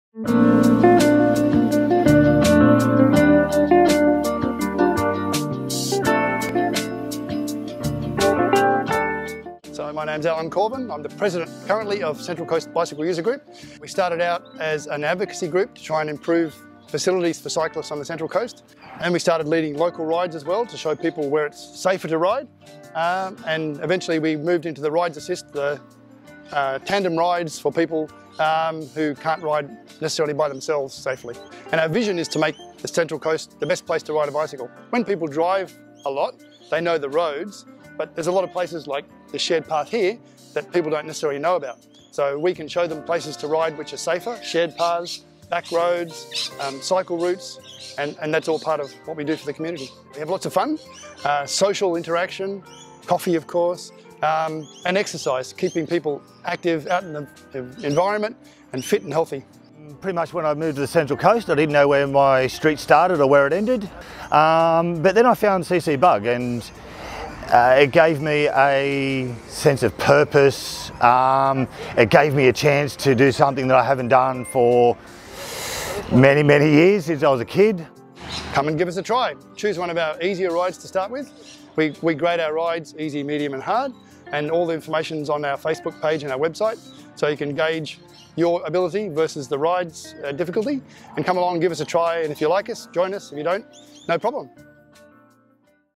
Central Coast Bicycle User Group
Central Coast Bicycle Users Group organise and lead regular social bicycle rides around the Central Coast which are open to all people of all ages and abilities.